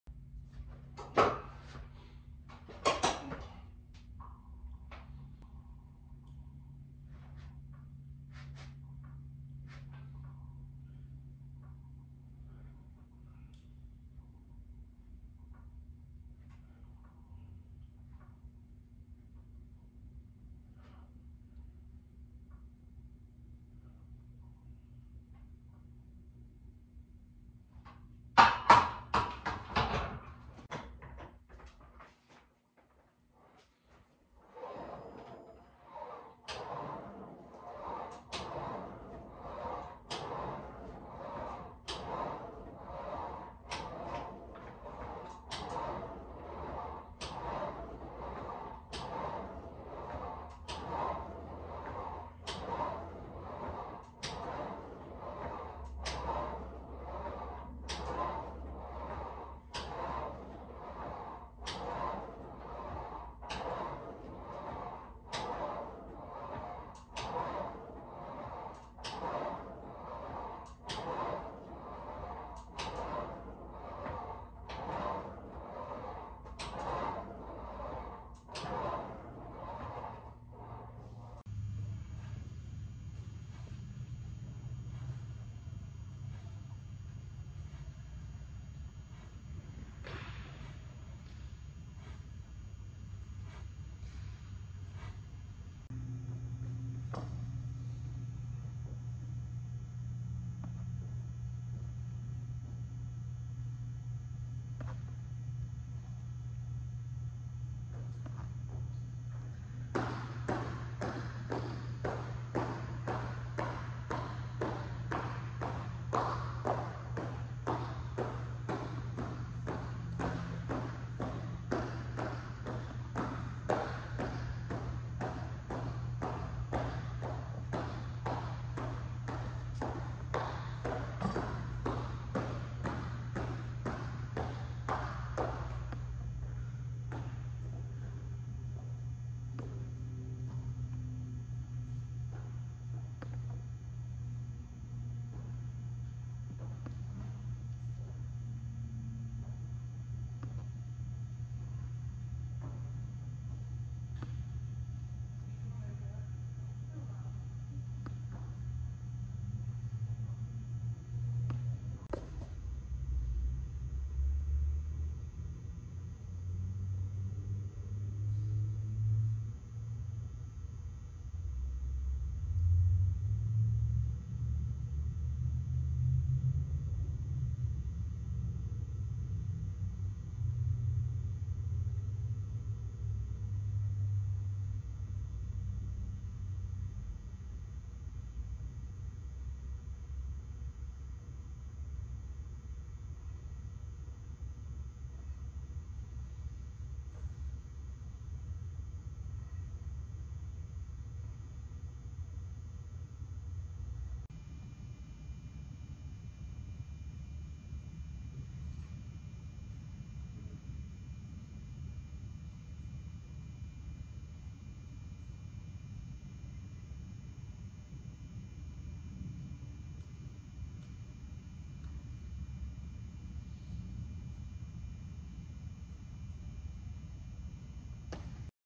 Full body workout to stay fit
Workout:

Barbell Bench Press: 15 reps
Row Machine: 25 reps
Dumbbell row: 10 reps
Lateral step up: 8 each leg
Dumbbell side lunges: 20 reps
Leg up: 12 reps

Repeat 4-5 times
LETS WORK! Another day to get mentally and physically better... Train hard, train smart and eat healthy. Be the example the words to see...